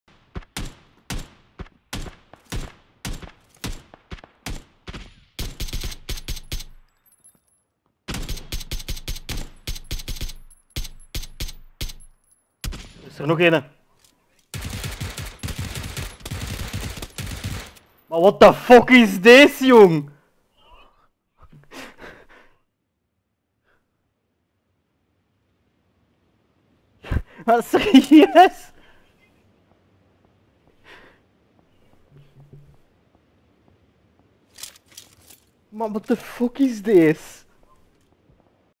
My god Kevin wtf
Squaddie goes super stealthy and sneaks up a squad.